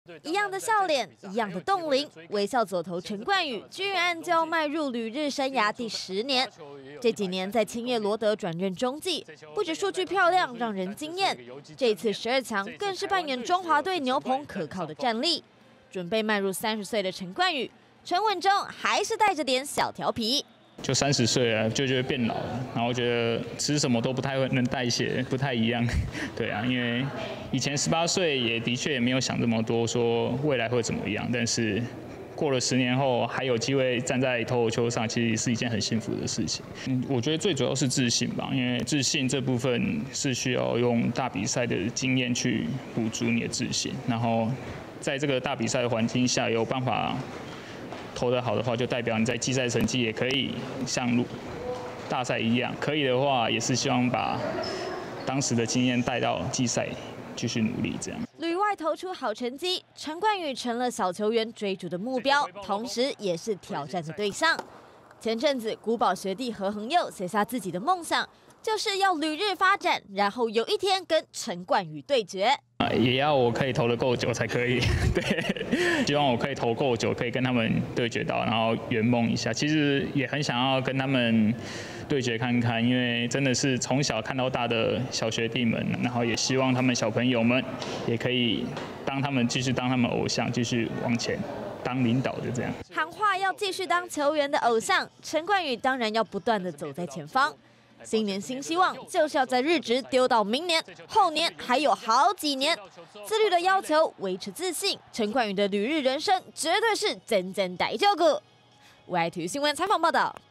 12/23 旅日人生邁向10年 陳冠宇能量滿點
4月開打的六搶一，誰來辦終於有個定案，但適逢球季中，選手的徵招就比較難掌握。12強表現出色的旅日左投陳冠宇，先前也表示能不能打還是得看球團意願。

尤其放眼新球季，陳冠宇不只要邁向30歲，更要展開自己的旅日第10年，今年有著滿滿收穫的他，不只有新年新希望，也自信喊話，要繼續當小朋友的偶像!